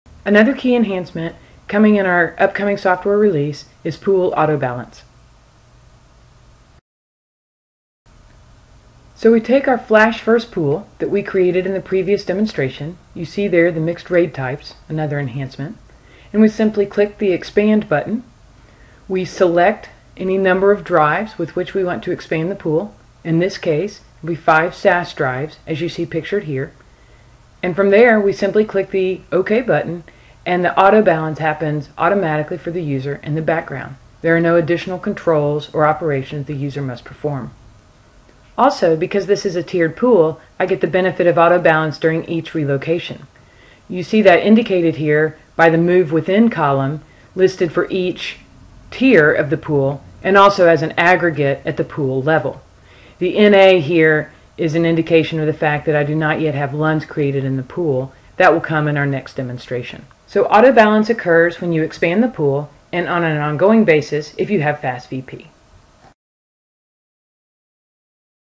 EMC World 2012 - Demo of VNX "Inyo" Storage Pool Autobalance
This is a quick demonstration of the new feature for VNX storage pools in the "Inyo" release which makes it simple and easy to add additional IOPs and/or capacity to an existing pool.